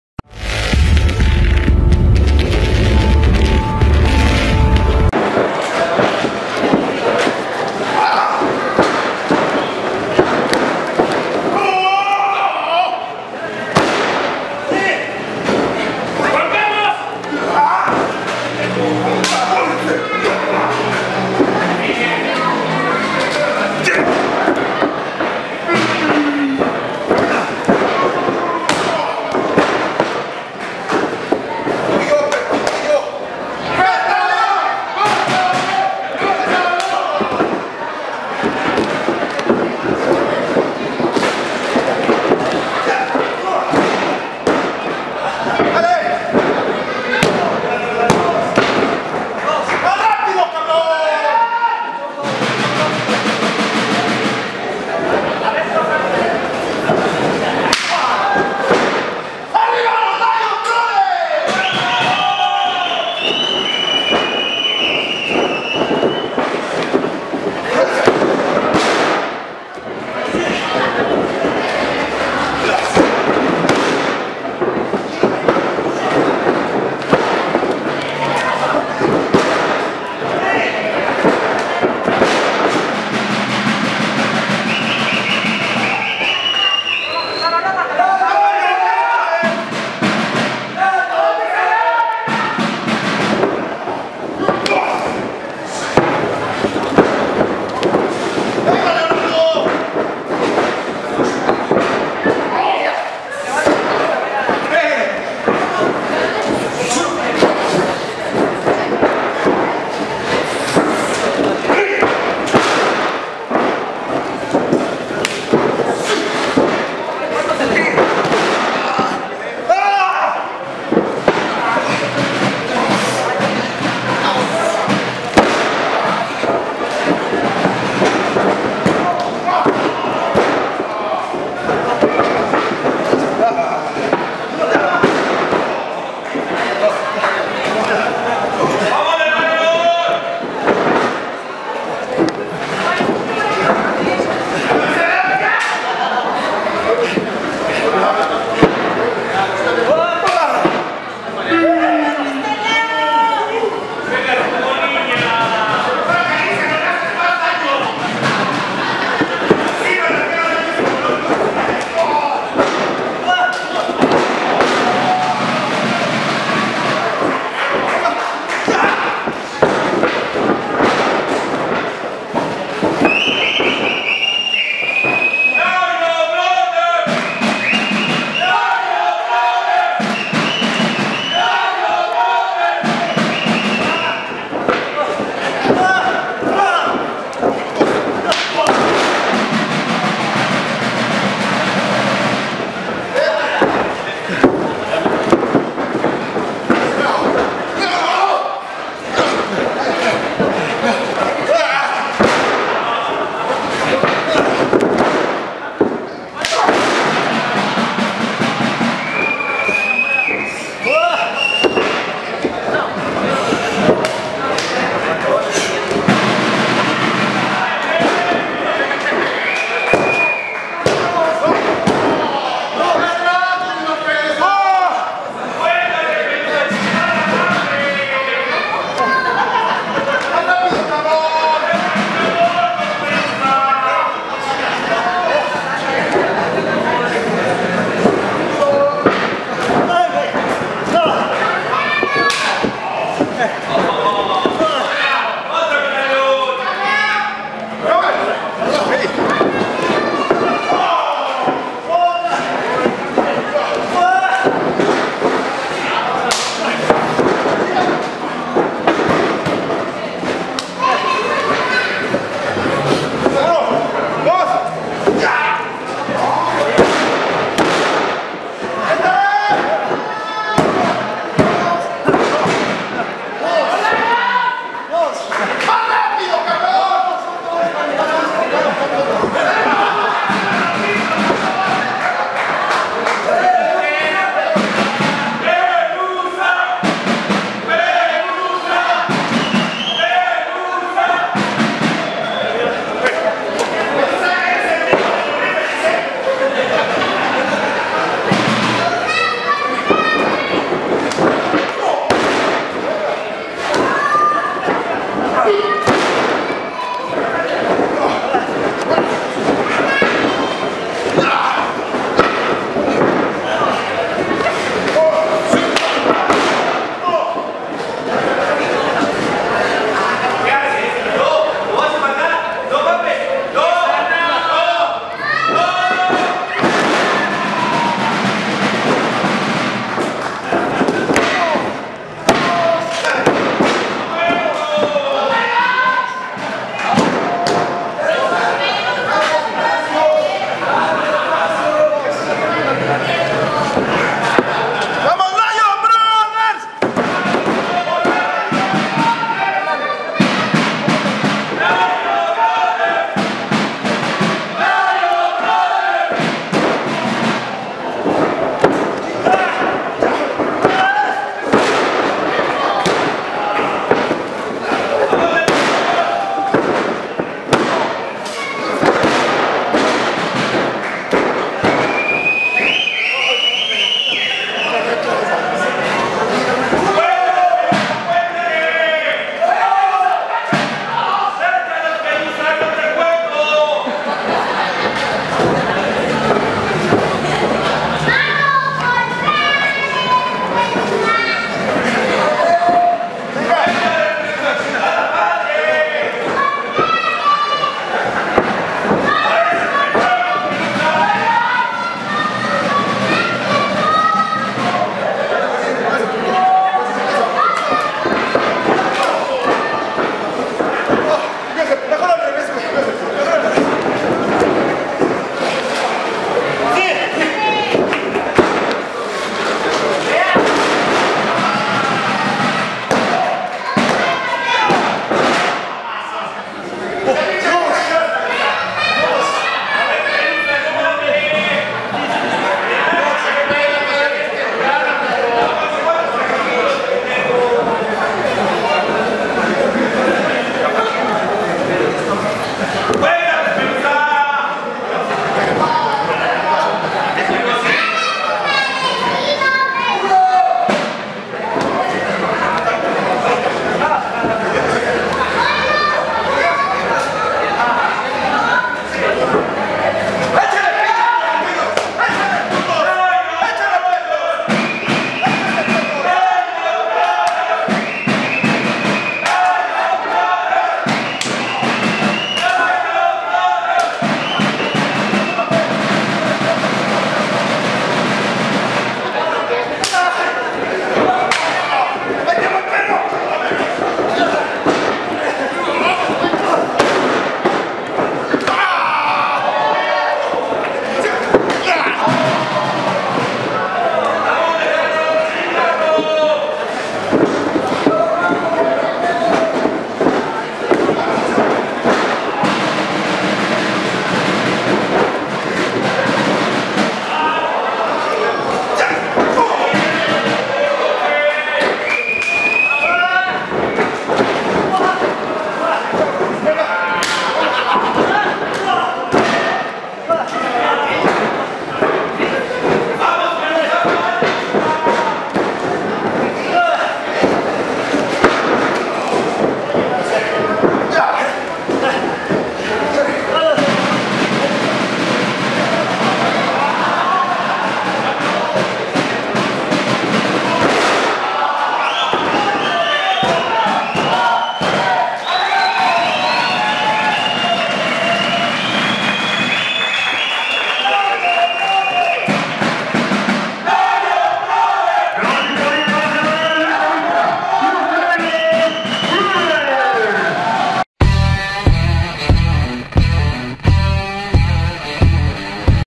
Metaleón y Mr. Leo vs Resplandor Jr y Sangre Felina en el Car de The Crash con IAW
Lucha en relevos sencillos en el Car de The Crash con IAW y RIW entre Metaleón y Mr. Leo vs Resplandor Jr y Sangre Felina.